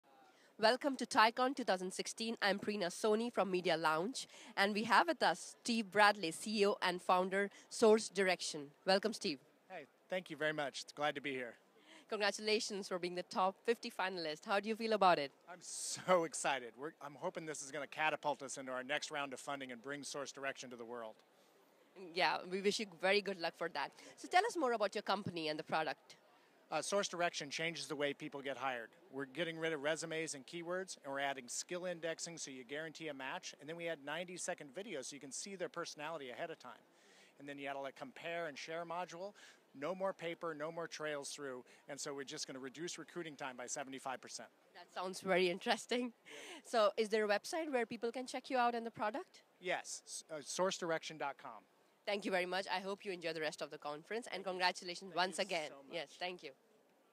Steve Bradley Interview at TiETV Lounge ooneeapp @TiEcon 2016
Interview recorded by TiETV Lite at the TiEcon 2016 Media Lounge.